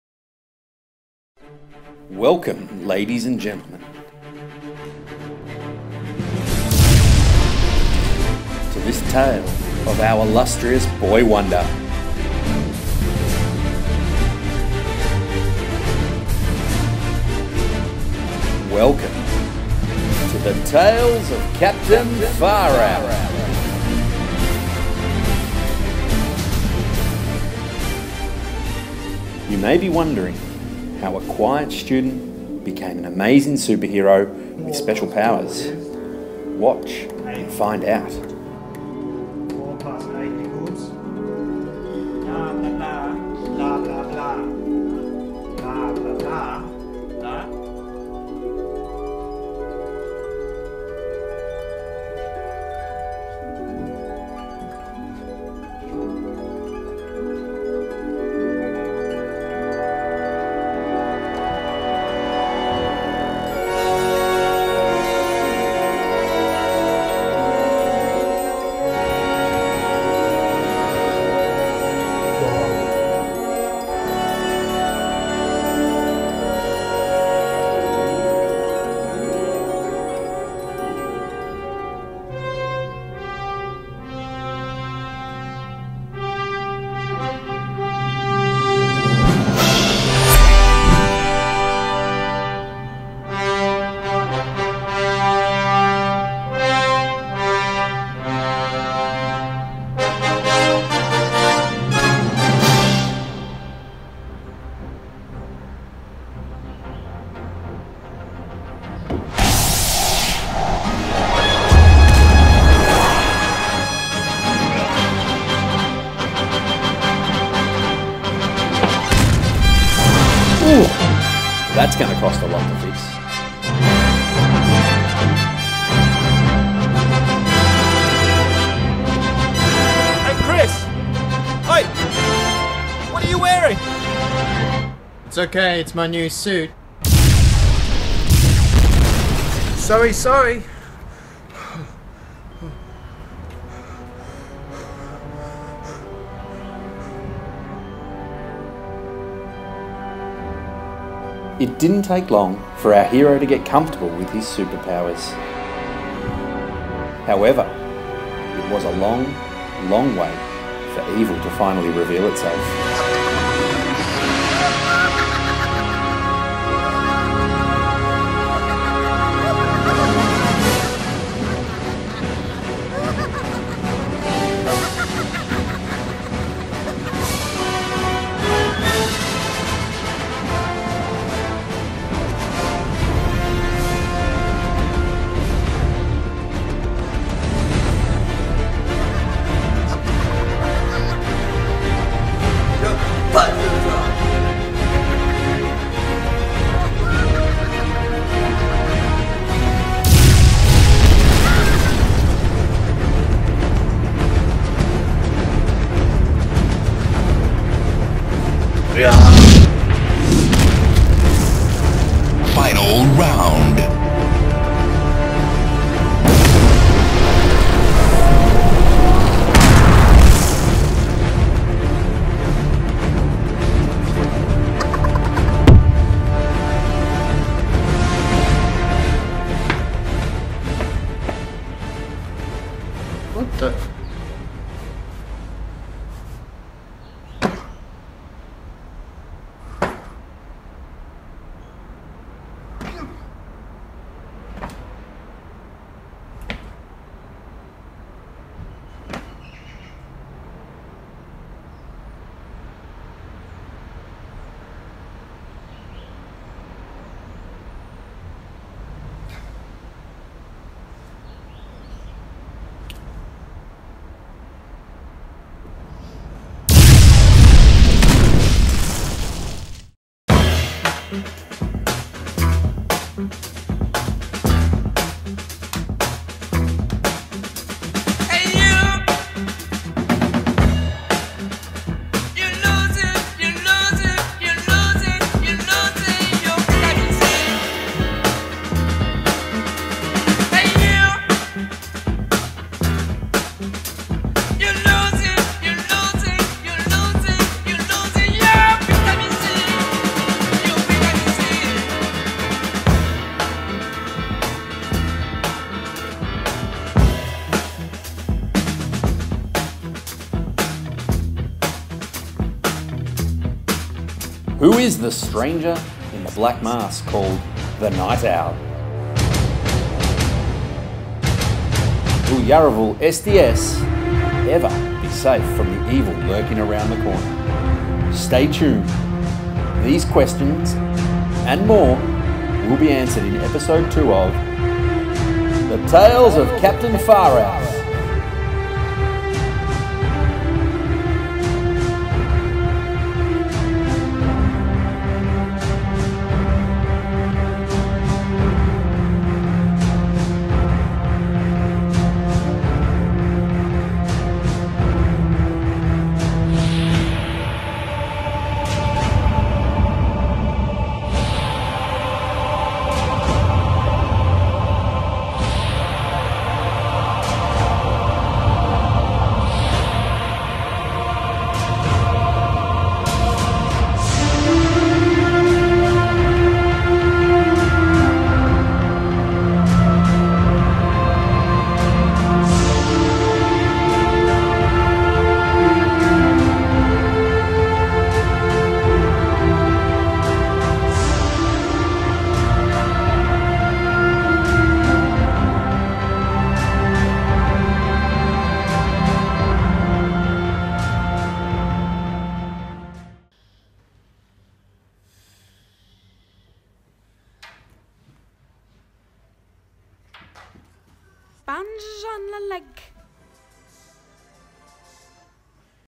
The Tales of Captain Far-Out
Created by students at Yarraville Special Developmental School, as part of their Photography Electives Group.
Term 3. September. 2017.

Shot entirely on an iPhone 5s.
Post Production in Adobe Premiere Pro.

This is an Unlisted link. Please consider this before sharing.